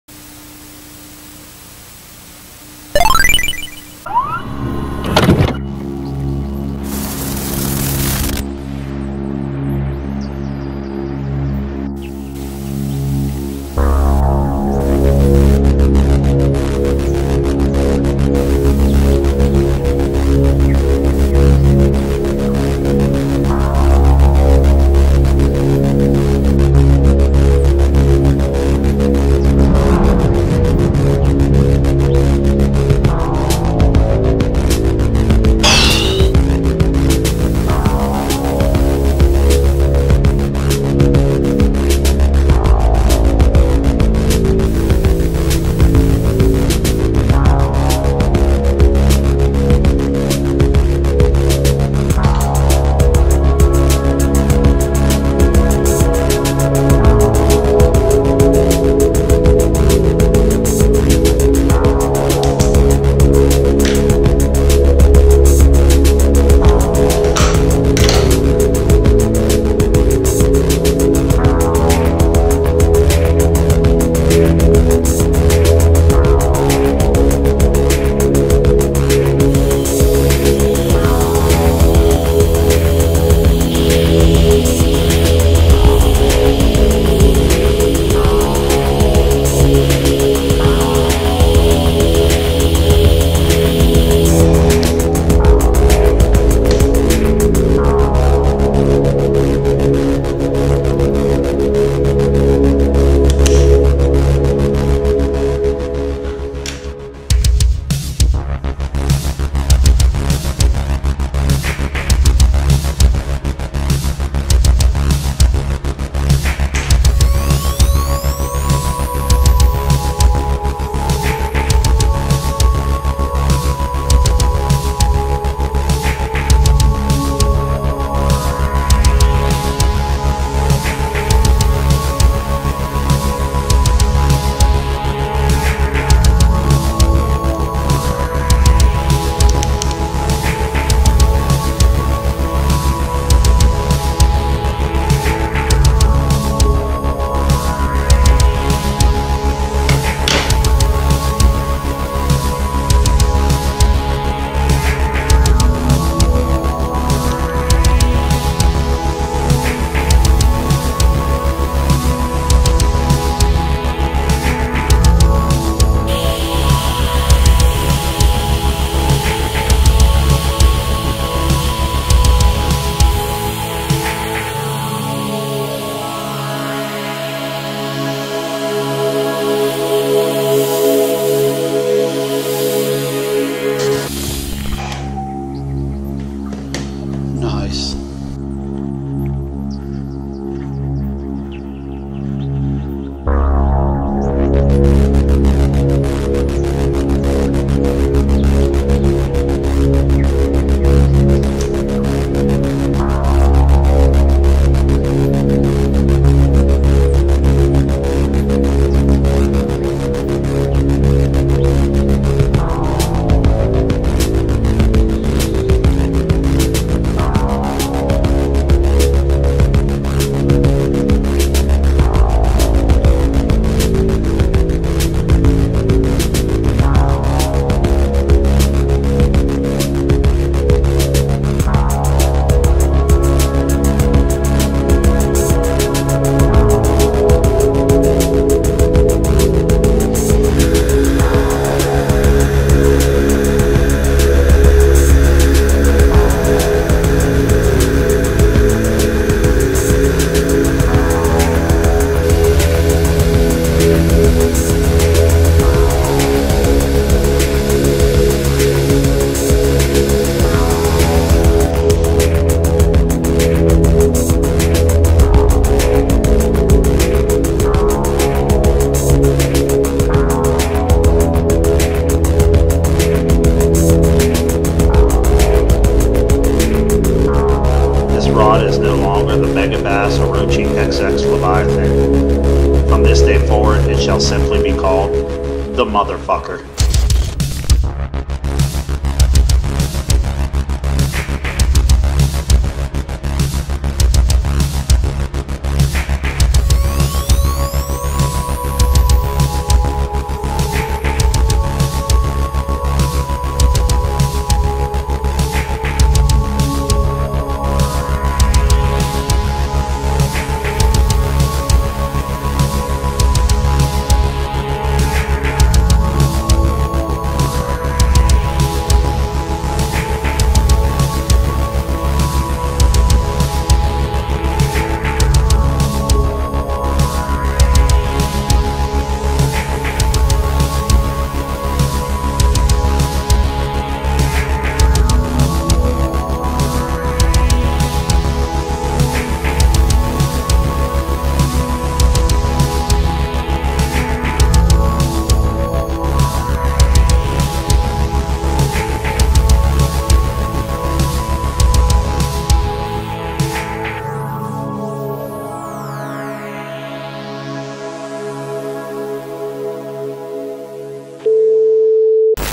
DIY Megabass Orochi XX Leviathan Heat Shrink Tubing Grip Rod Handle Gen 1 Gen 2 X Flock Cork
DIY Megabass Orochi XX Leviathan Heat Shrink Tubing Grip Rod Handle Gen 1 Gen 2 X Flock

This is not a tutorial, first time I ever used this product and it turned out insanely awesome.